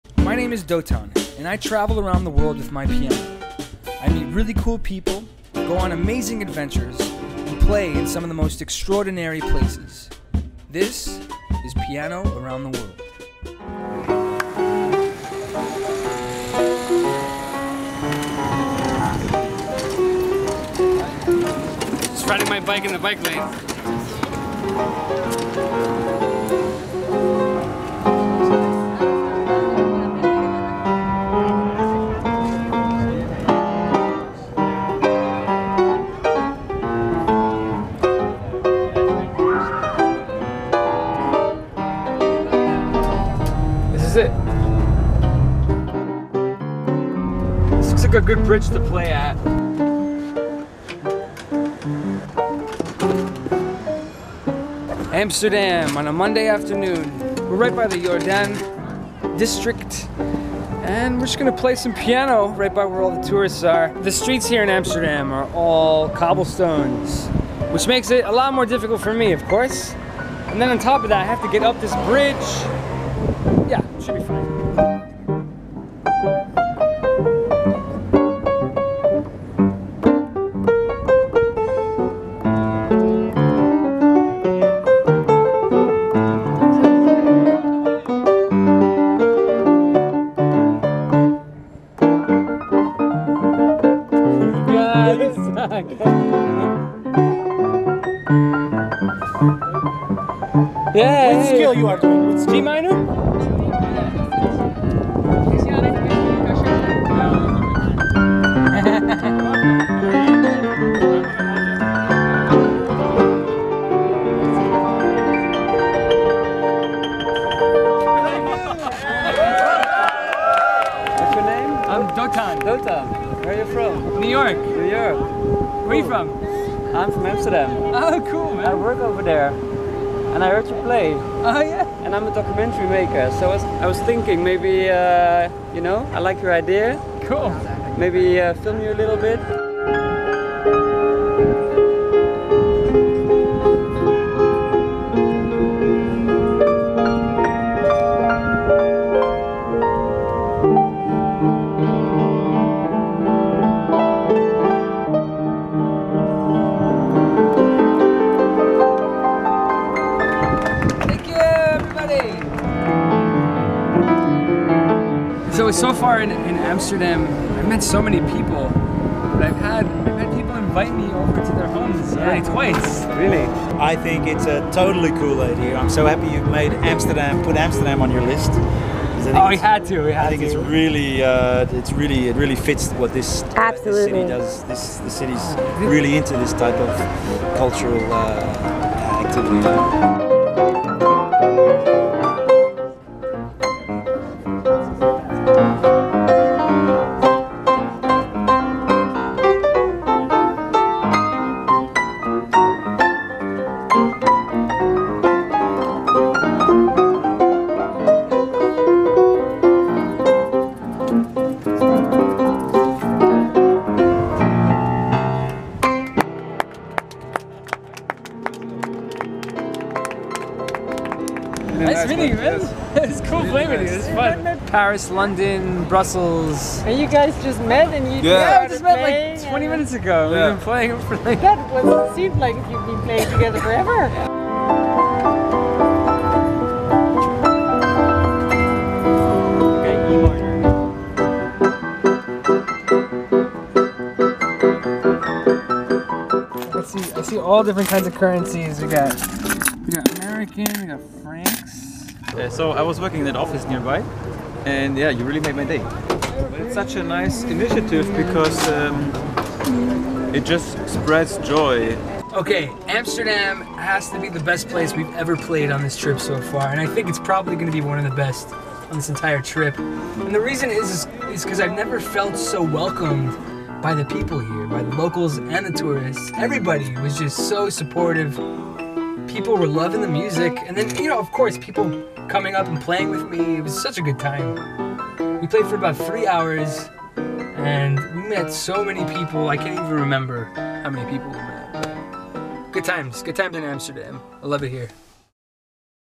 Best place to Street Perform in Amsterdam | Europe Episode #21
Amsterdam is an incredible city to busk in and when I arrived I had trouble finding a good place to set up my piano on the streets. I found this little bridge over a canal that was beautiful, parked the van, and played for 4 hours. It turned out to be a blast and we met so many people!!

Live an Extraordinary Life!